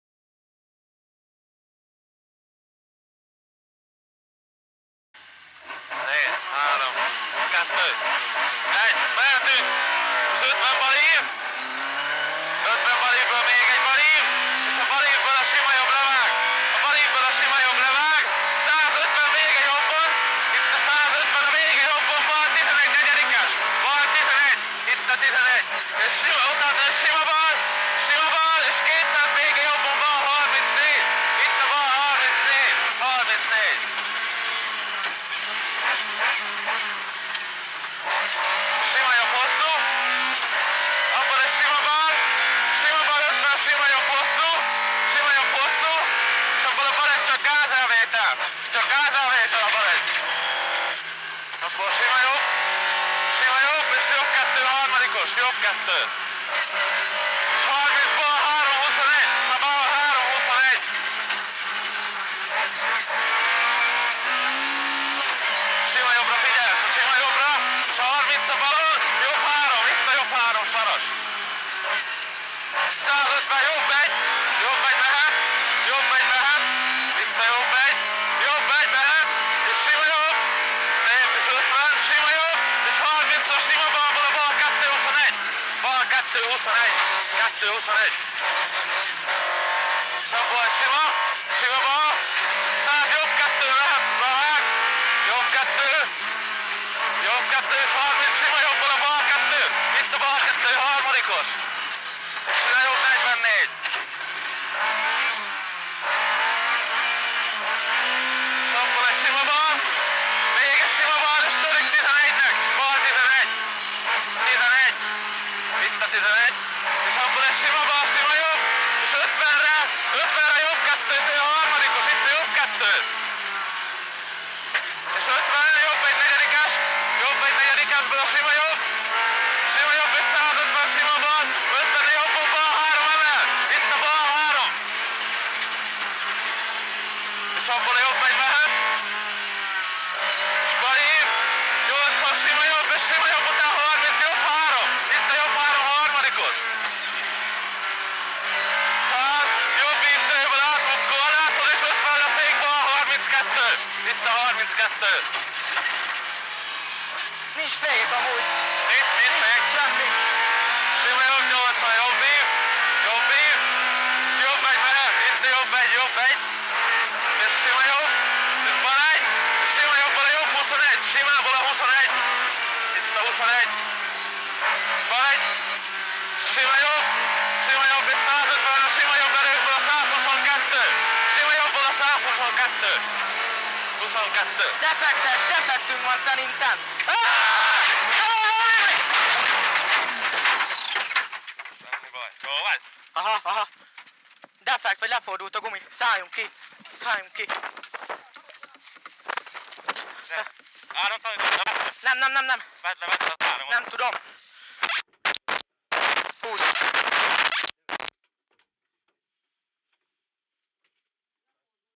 Szilágyi Géza - Berényi Gábor Bükkszentkereszt CRASH
Egy műszaki hiba a verseny végét jelentette.